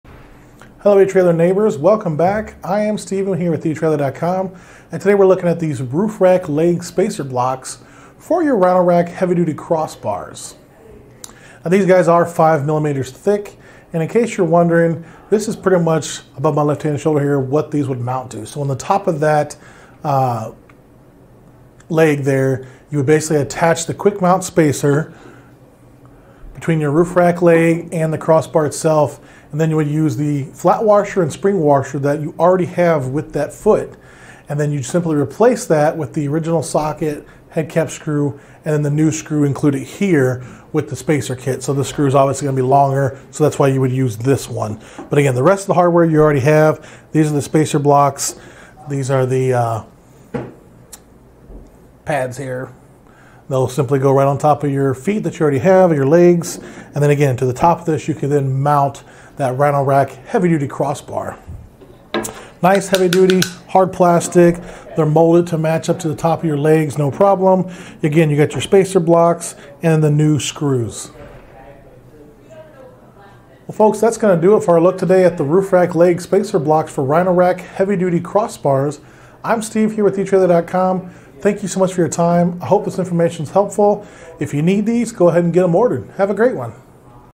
etrailer | Hands-On with the Roof Rack Leg Spacer Blocks for Rhino-Rack Heavy-Duty Crossbars
Hello etrailer neighbors.  Welcome back.  I am Steven here with etrailer.com,  and today we're looking at these roof rack leg spacer blocks  for your Rhino-Rack heavy duty crossbars.  Now these guys are 5 millimeters thick,  and in case you're wondering, this is pretty much  above my left hand shoulder here, what these would mount to.  So on the top of that leg there  you would basically attach the quick mount spacer  between your roof rack leg and the crossbar itself.  And then you would use the flat washer  and spring washer that you already have with that foot.

And then you'd simply replace that  with the original socket head cap screw  and then the new screw included here with the spacer kit.  So this screw's obviously gonna be longer,  so that's why you would use this one.  But again, the rest of the hardware you already have,  these are the spacer blocks.  These are the pads here.  They'll simply go right on top of your feet  that you already have or your legs,  and then again, to the top of this, you can then mount  that Rhino-Rack heavy duty crossbar.

Nice heavy duty, hard plastic.  They're molded to match up to the top  of your legs no problem.  Again, you got your spacer blocks and the new screws.  Well, folks, that's gonna do it for our look today  at the roof rack leg spacer blocks  for Rhino-Rack heavy duty crossbars.  I'm Steve here with etrailer.com.

Thank you so much for your time.  I hope this information's helpful.  If you need these, go ahead and get 'em ordered.  Have a great one.